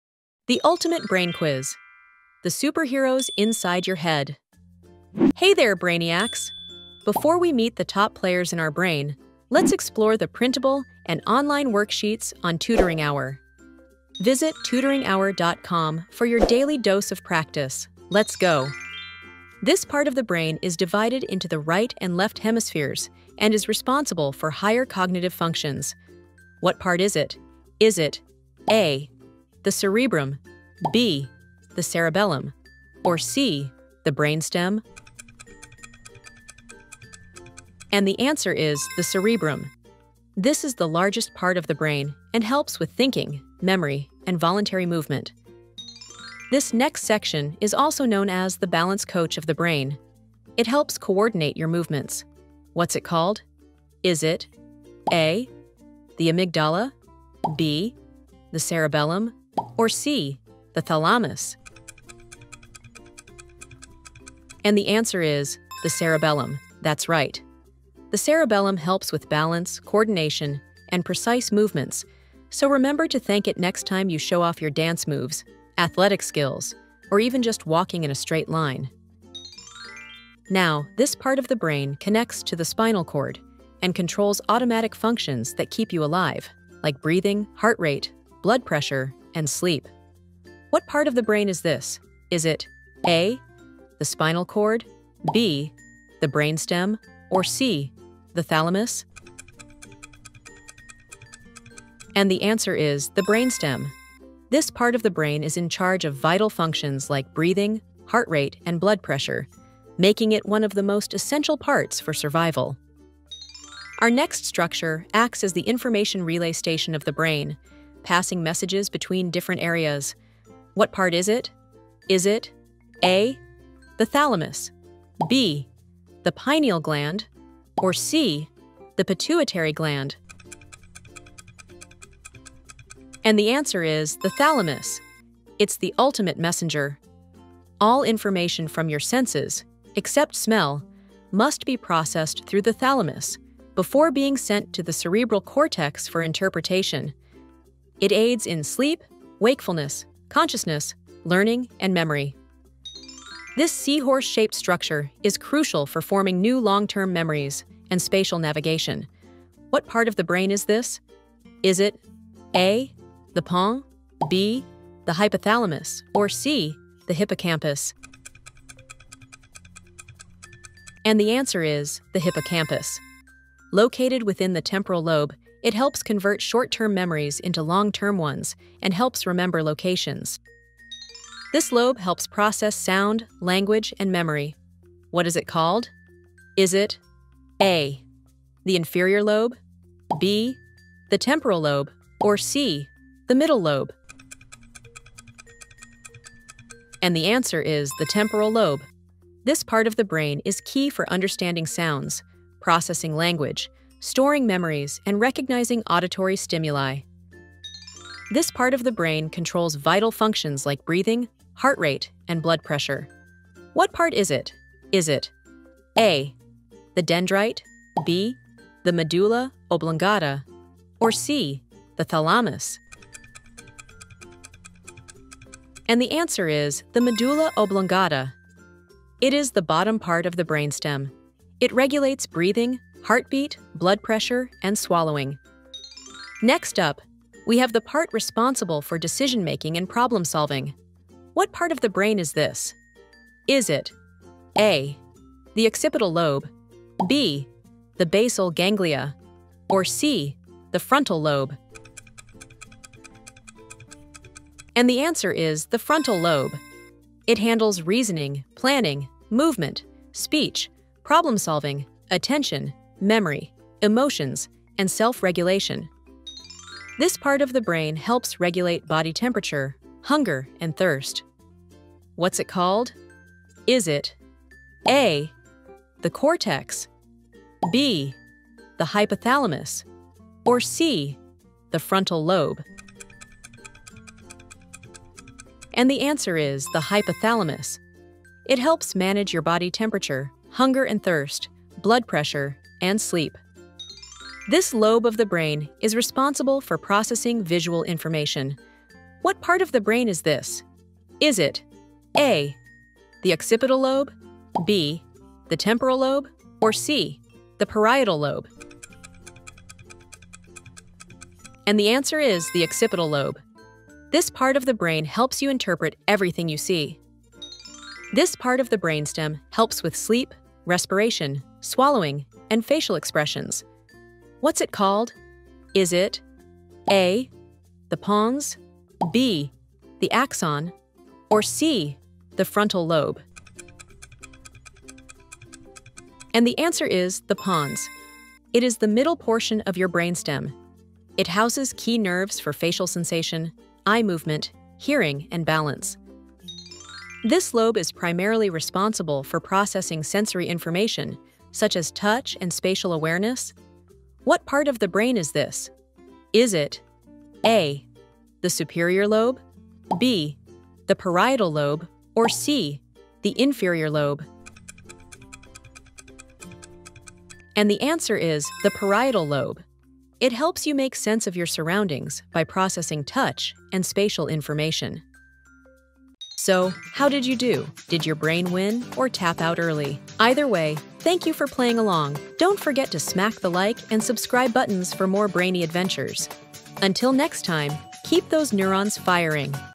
The Human Brain Structure and Functions Quiz | Meet the Superheroes of Your Brain!
Think you have a sharp mind? Put your brainpower to the test with this fun and exciting human brain structure and functions quiz! From the main superheroes of the brain—the Cerebrum, Cerebellum, and Brainstem—to the vital Thalamus, memory-boosting Hippocampus, and the powerful Frontal, Temporal , Parietal, and Occipital lobes, we’re diving deep into what makes your brain tick. Ever wondered which part of your brain is responsible for memory? Or where vision is processed? This quiz will not only challenge your knowledge but also uncover the incredible functions of each brain region, helping you learn as you play. Whether you’re curious about how your brain controls movement, processes emotions, or keeps your body running smoothly, this quiz has it all. Get ready to explore the wonders of the human brain a way that’s both fun and educational!

00:19    The Cerebrum
00:46    The Cerebellum
01:20    The Brainstem
01:54    The Thalamus
02:33    The Hippocampus
03:01   The Temporal Lobe
03:28   The Medulla Oblongata
03:56   The Frontal Lobe
04:26   The Hypothalamus
04:54   The Occipital Lobe
05:18   The Pons
05:47   The Parietal Lobe